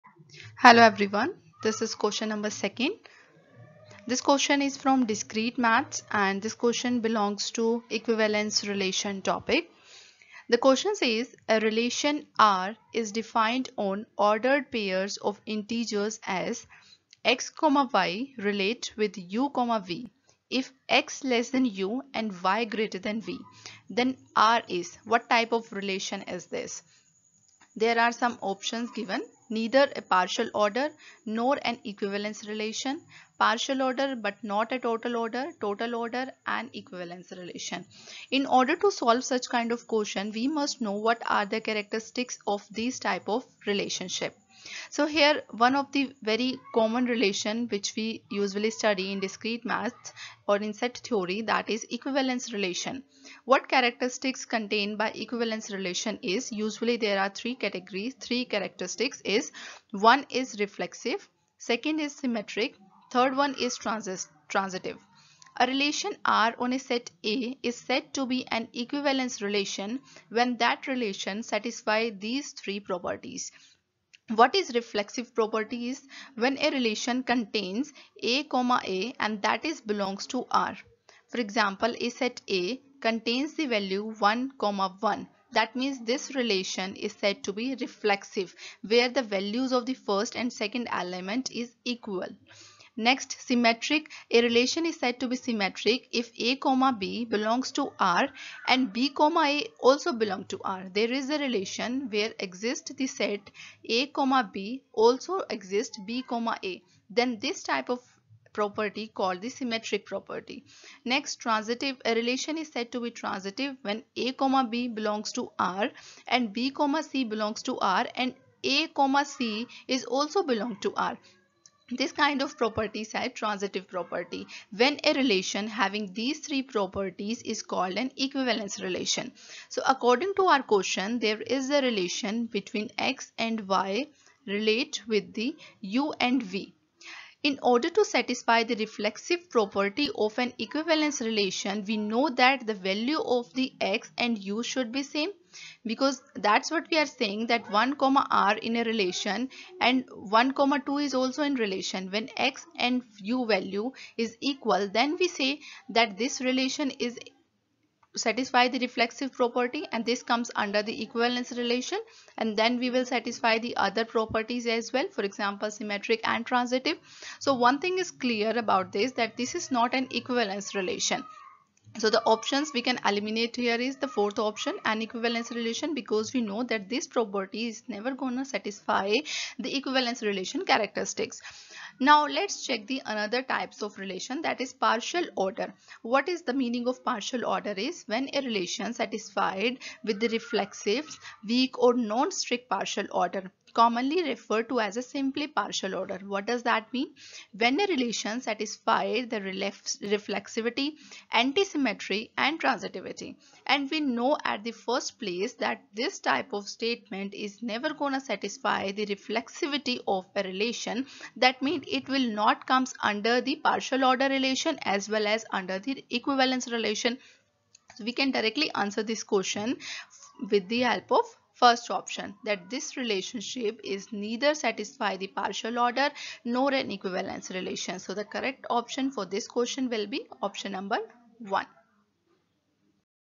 NTA UGC NET DEC-2022 PAPER-2 | EM | RELATIONS | UGC NET TEST SERIES |SOLUTIONS ADDA| EXP. BY NEERAJ
NTA UGC NET DEC-2022 PAPER-2 Q2:  A relation 'R' is defined on ordered pairs of integers as:
(x,y)R(u,v) if x LESSTHAN u and y GREATER THAN v. Then R is